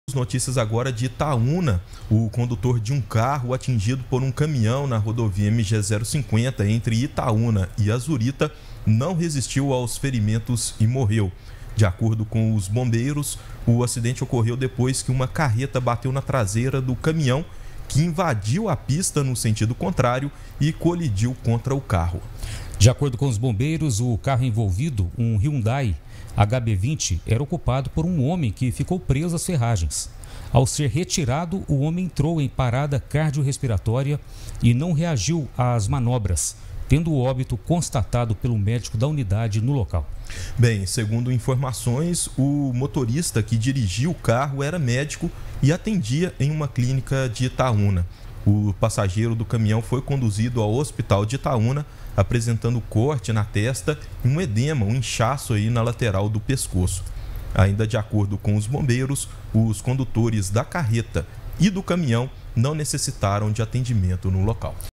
HOMEM MORRE EM ACIDENTE NA MG-050, EM ITAÚNA
| REGIÃO |

O condutor de um carro atingido por um caminhão, na MG-050, entre Itaúna e Azurita, não resistiu aos ferimentos e morreu. Saiba mais informações sobre o acidente: